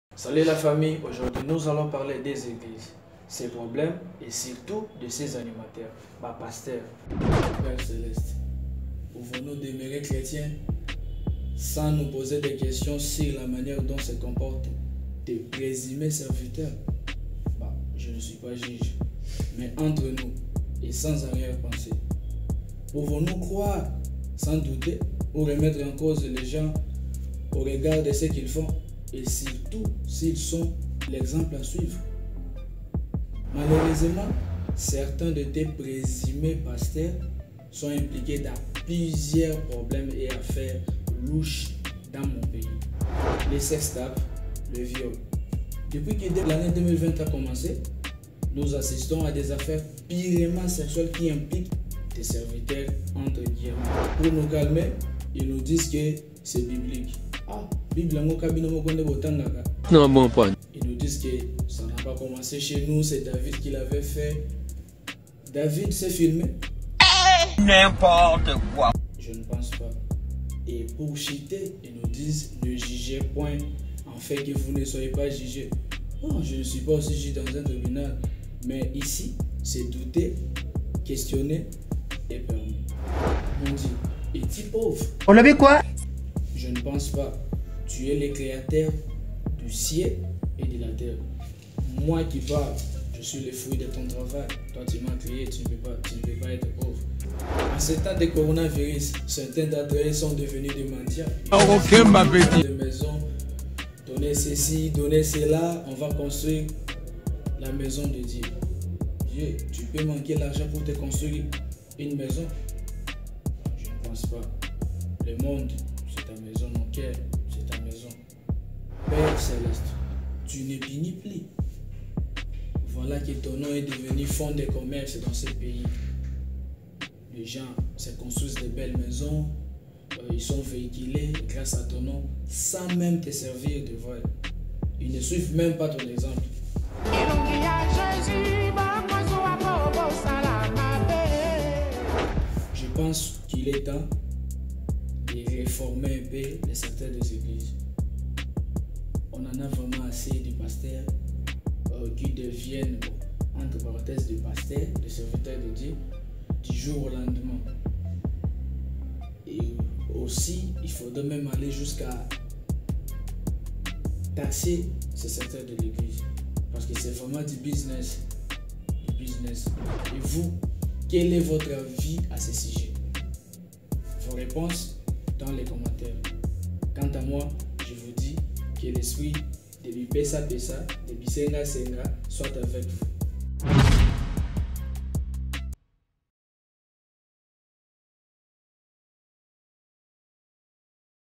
Silence, « l’homme de Dieu » parle !
Ces scandales au sein de nos ‘’églises’’… On en parle.